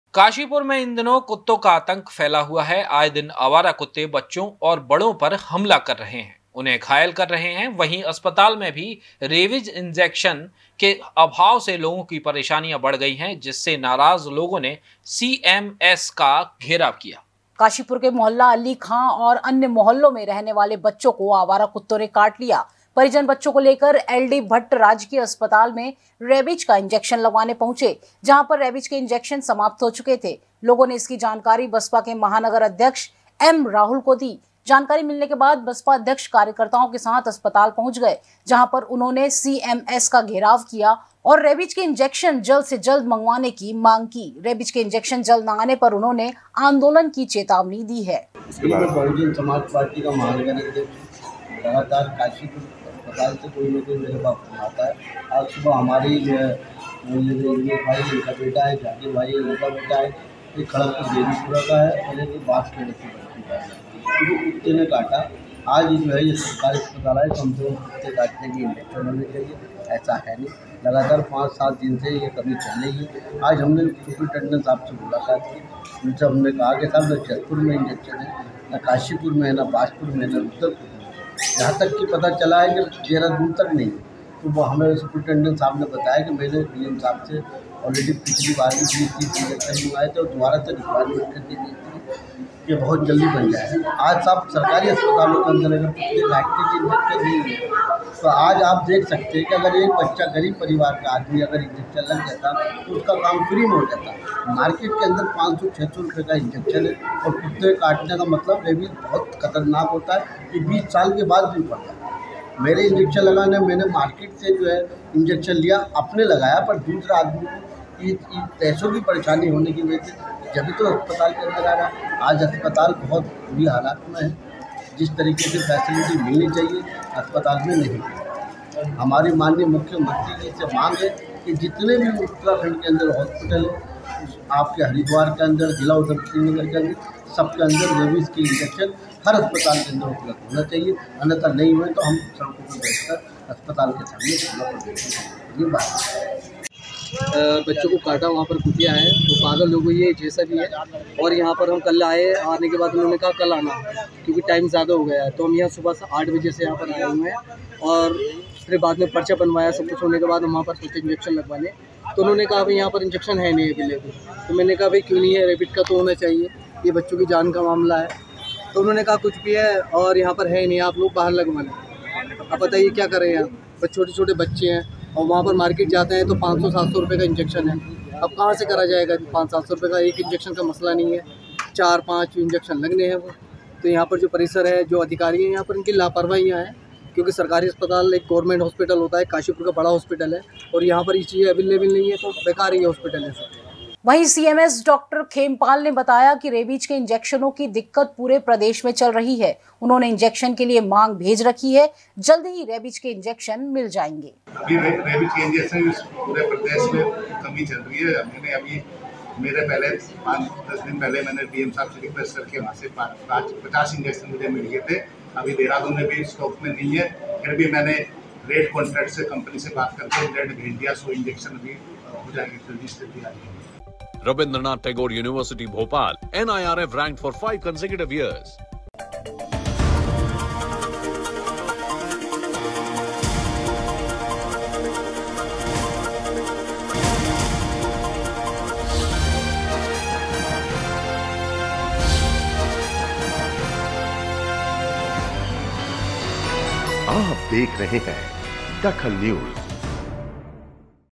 DAKHAL NEWS रेबिज इंजेक्शन के अभाव से बढ़ी लोगों की परेशानी
रेबिज इंजेक्शन के अभाव से बढ़ी लोगों की परेशानी
नाराज लोगों ने सीएमएस का घेराव किया